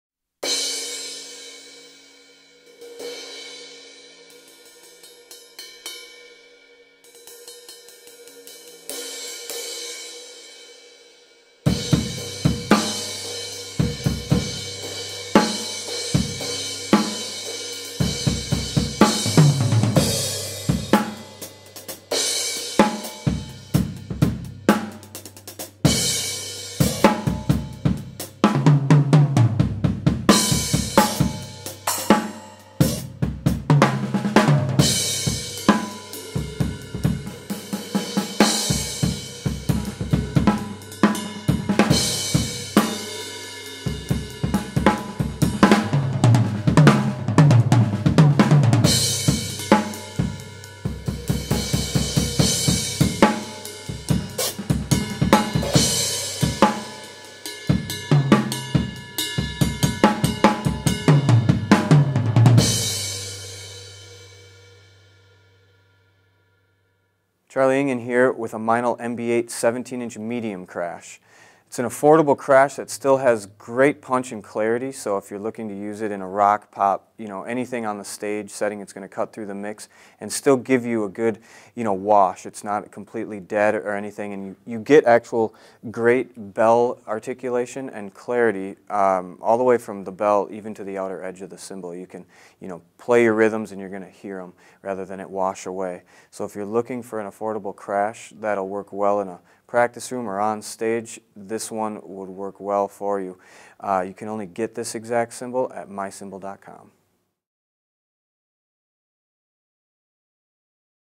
Meinl 17" Mb8 Medium Crash Cymbal - 1270g - Played by Charlie Engen (MB8-17MC-B-209Y)
Other gear used in this video:
• Cymbals (From drummer's L to R): Meinl 13" Mb8 Medium Hi Hat Cymbals, Meinl 16" Mb8 Heavy Crash Cymbal, Meinl 10" Mb8 Splash Cymbal, Meinl 20" Mb8 Medium Ride Cymbal